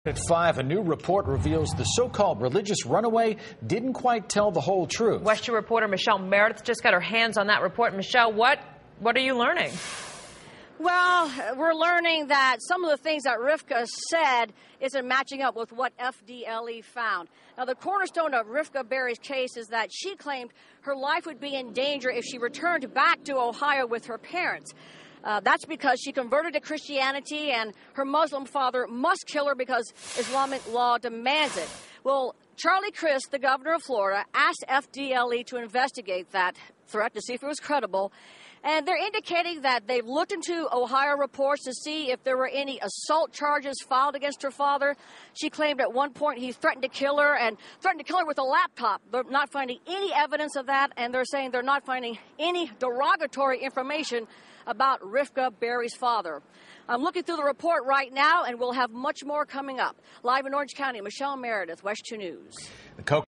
FDLE: No Evidence Of Religious Runaway's Claims
A report from the Florida Department of Law does not support claims made by a teenage runaway that her Muslim father would kill her for converting to Christianity.